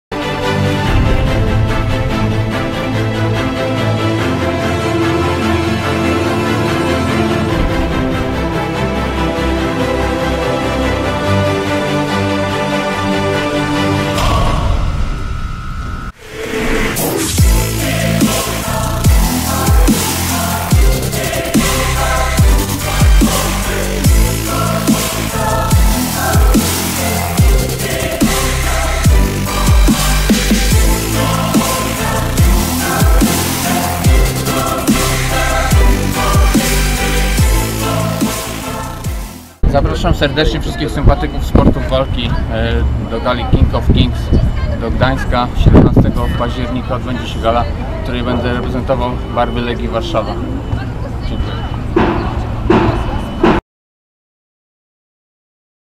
Przemysław Ziemnicki before King of Kings in Gdańsk, Poland
Traning in Garuda Gym, Warsaw, Poland

Video: Lukas Czarnowski